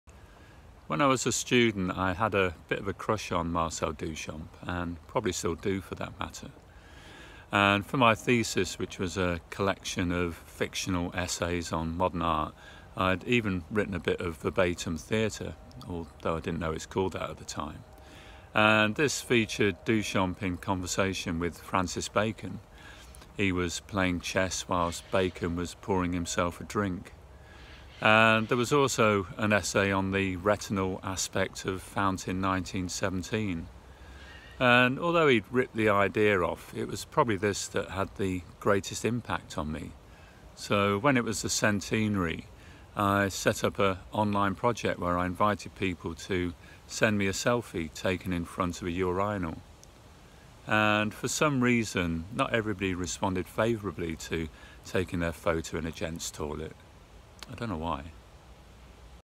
one minute lecture no 41 : fountain 2017
From a series of one minute lectures produced to  accompany his retrospective exhibition, From Now Until Yesterday, Tony Baker talks about his crush on Marcel Duchamp and his project Fountain 2017 where he invited people to submit photographs of themselves in front of a urinal to celebrate the centenary of Fountain 1917

To view his work, go to :
web: tonykaraoke .wix.com/work